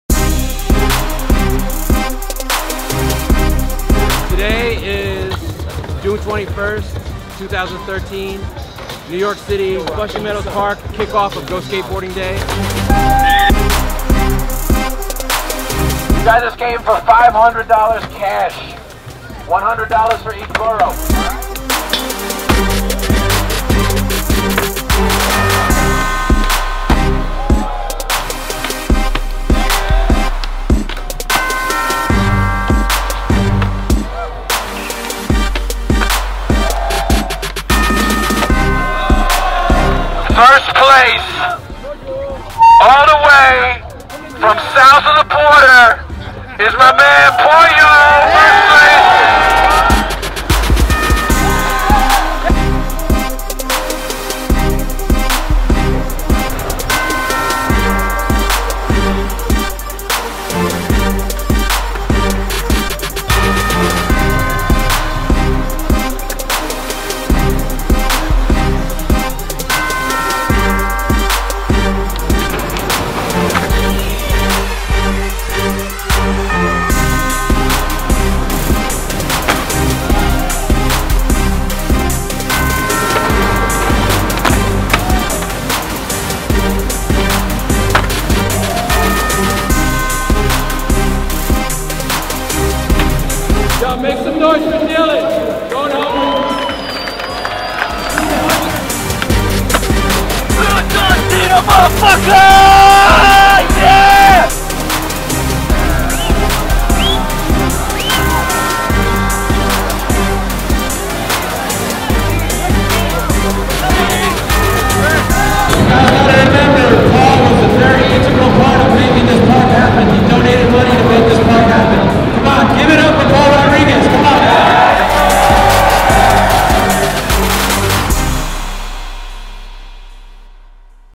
Go Skateboarding Day NYC 2013 | Video Recap
Go Skateboarding Day New York 2013 is officially a wrap and with it concluded yet another incredible day where thousands of the city's local skateboarders come together for a massive all city session. Kicking off as planned at Flushing Meadows skatepark with a best trick event on the park handrail as well as tons of giveaways and prizes from Nike SB, 5boro and CCS, the GSD crew gathered in hundreds in and around the park to watch and skate. Lead by legendary NYC skateboard ambassador Steve Rodriguez the crew hit the subway and headed back to the city for the traditional push across the Williamsburg. Hundreds of skaters were paid a surprise visit on the bridge from Nike SB teamriders Paul Rodriguez, Alex Olson, Stefan Janoski, Shane O'Neill, Gino Iannucci, Weiger Van Wagenen, Ishod Wair and more who pushed alongside them into the city's Lower Eastside.
Then it was off to the LES Coleman Playground where things really heated up. The park was packed to the gills and included best trick events, prize giveaways, a live DJ and another best trick jam presented by Nike SB and CCS.